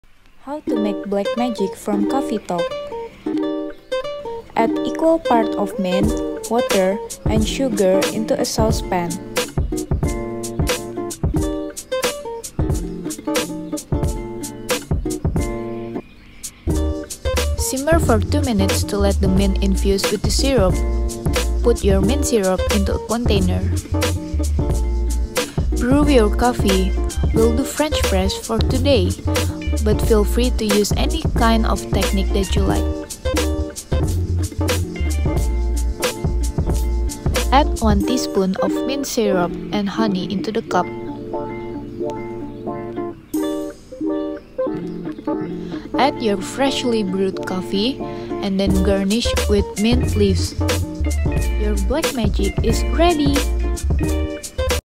How to make Black Magic from Coffee Talk!
Unique coffee with touch of mint and honey! ☕️ What should we make next? 👀